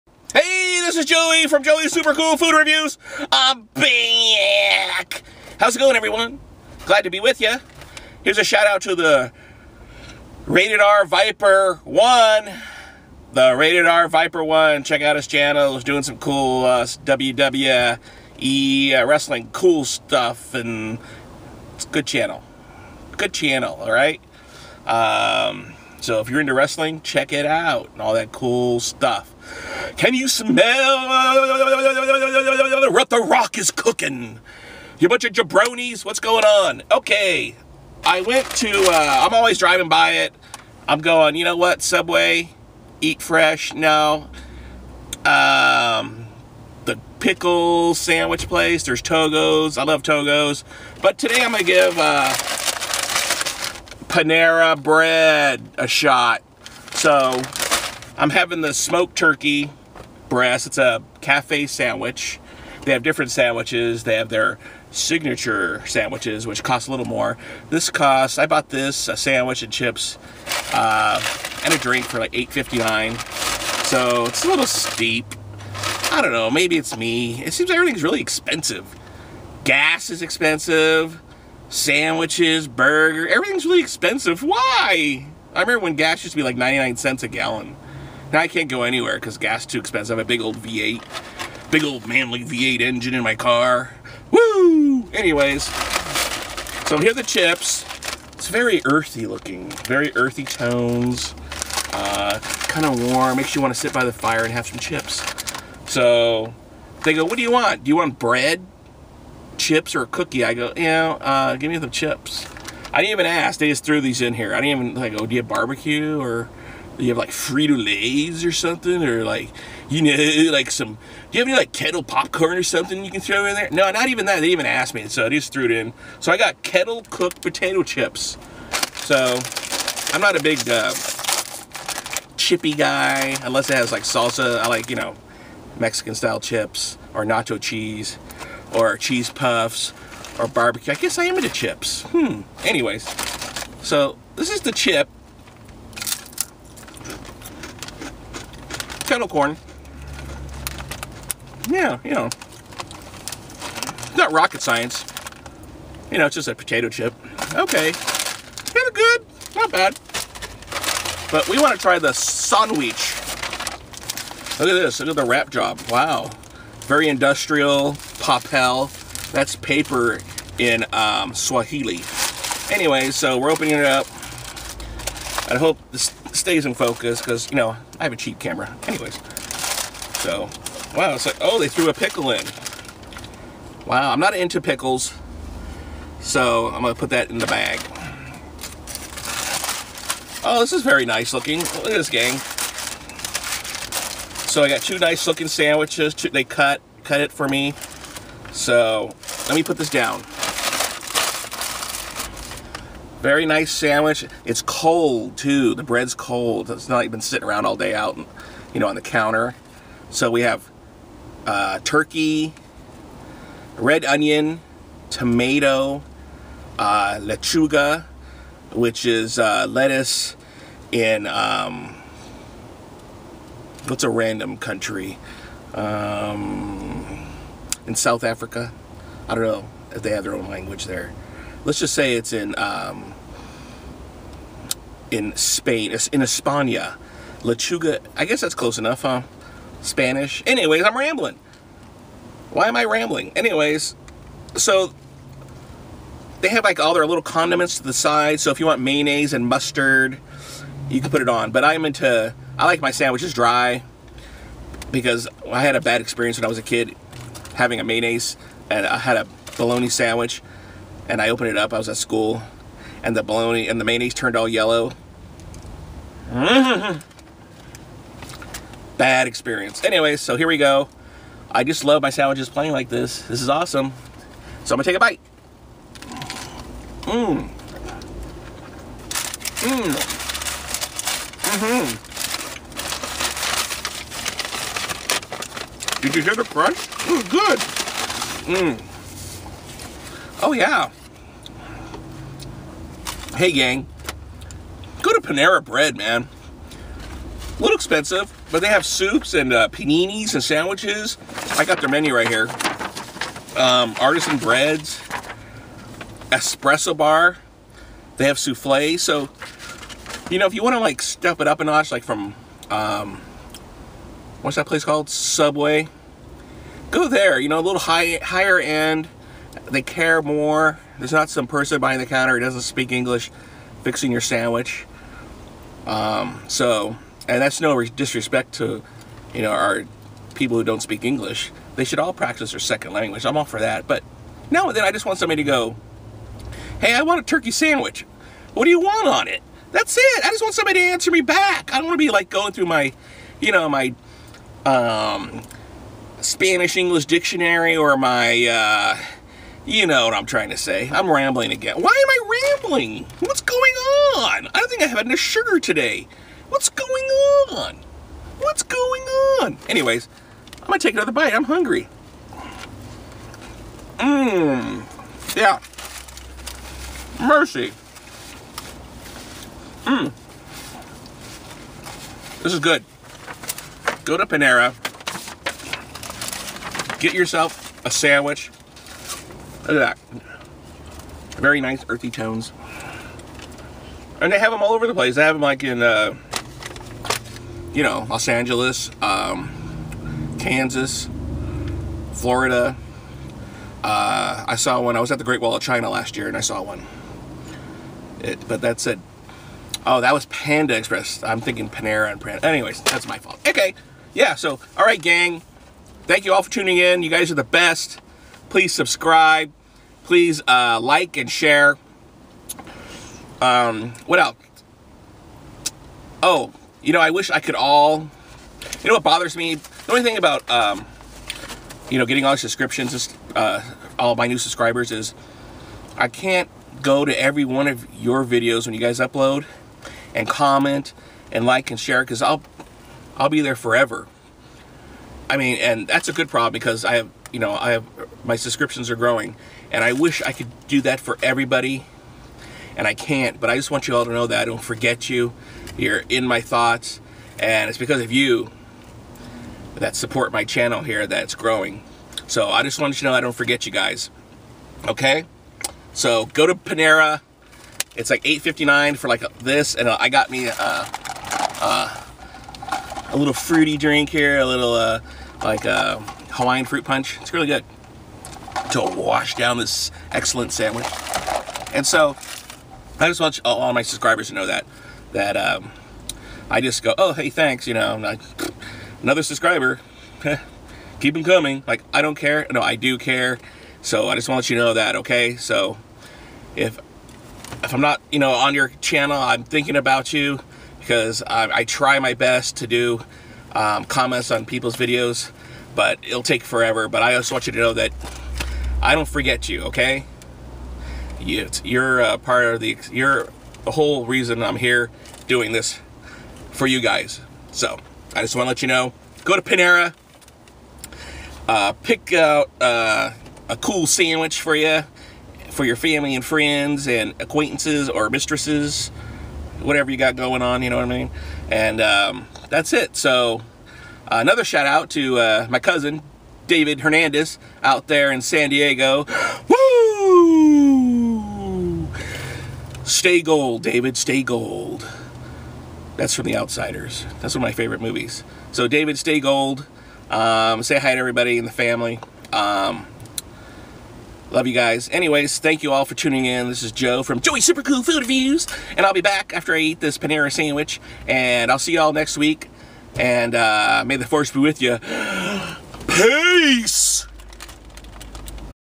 Panera Bread's Cafe Sandwich-SMOKED TURKEY BREAST REVIEW!!
Panera Bread's Smoked Turkey Breast Sandwich Review!! 99% fat-free smoked turkey breast, leaf lettuce, tomatoes, red onions and salt and pepper, all served on country white bread. $8.59 with a Large Drink.